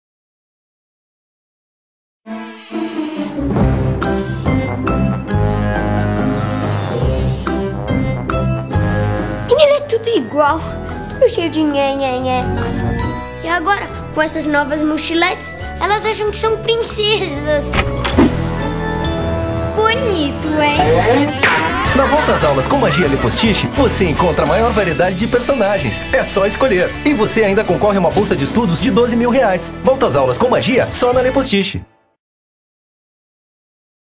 Le Postiche - Princesas Disney
Filme publicitário criado e produzido para a Le Postiche pela agencia FAM.